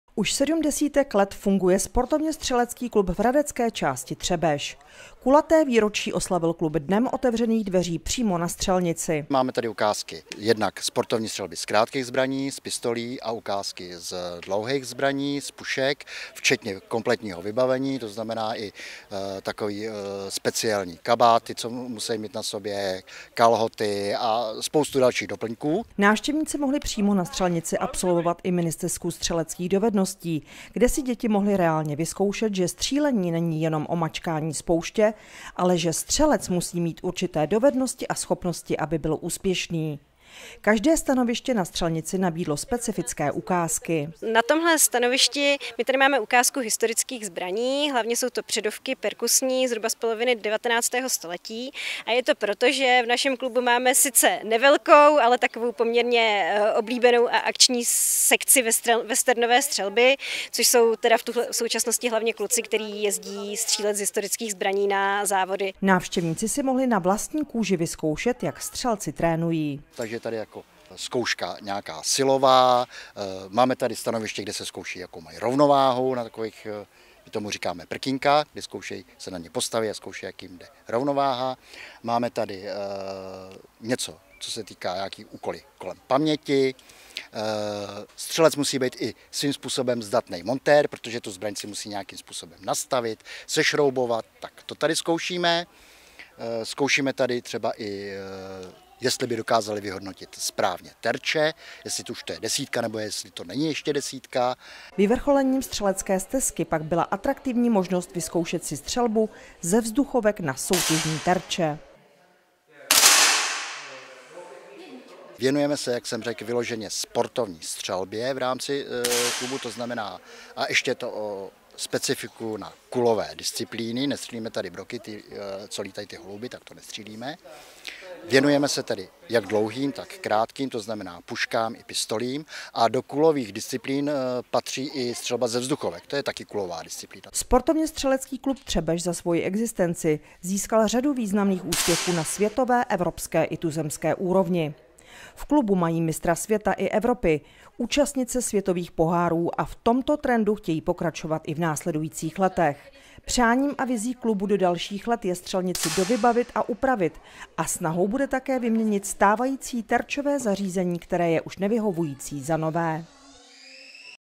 Sportovně střelecký klub Třebeš oslavil 70. výročí existence
Ukázky olympijské sportovní střelby, westernové střílení nebo střelba ze vzduchovek na závodní elektrické terče. To všechno a mnohé další bylo připraveno pro malé i dospělé zájemce na třebešské střelnici. Sportovně střelecký klub Třebeš se otevřel veřejnosti a oslavil 70. výročí svého založení.